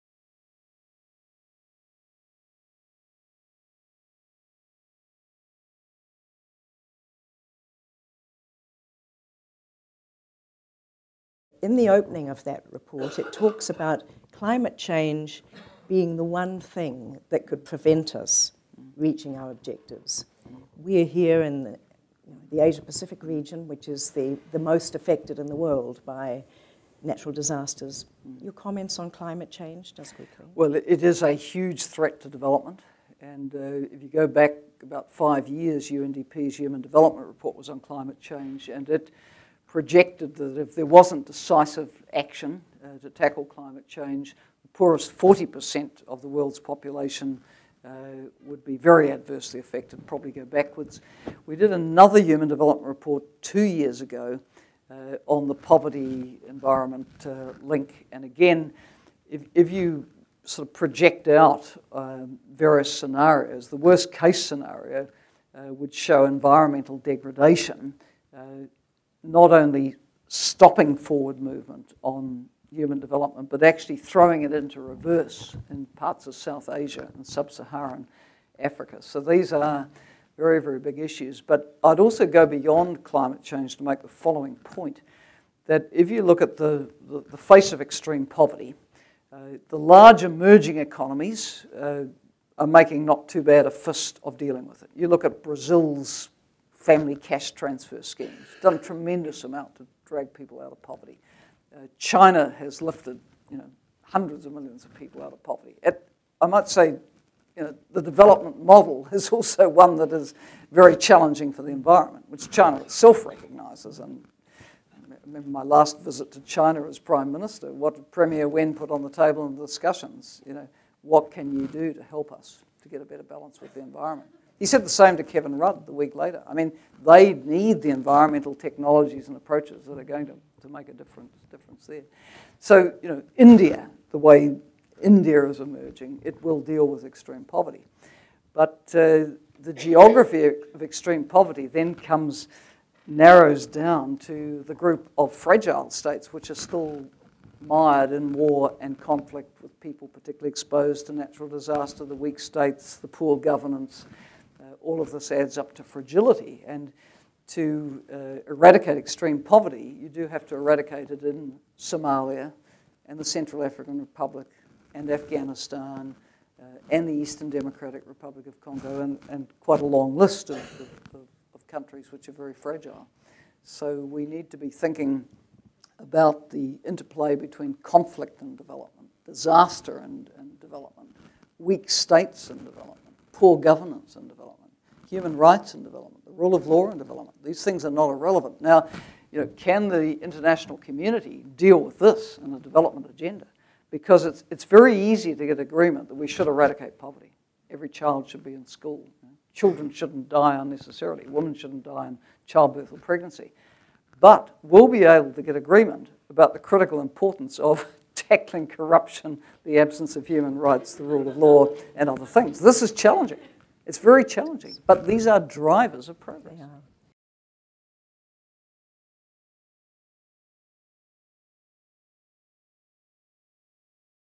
4 of 6 Gill Greer in Conversation with Helen Clark: Live Below the Line 2013
The Future We Want: The Millennium Development Goals and Beyond.

QUESTION: Can climate change prevent us from achieving our objectives?

As part of the buildup to Live Below the Line 2013 VSA CEO Gill Greer talks with Helen Clark about the future of development. This talk took place at the Spectrum Theatre in Wellington, New Zealand, on 22 August 2013.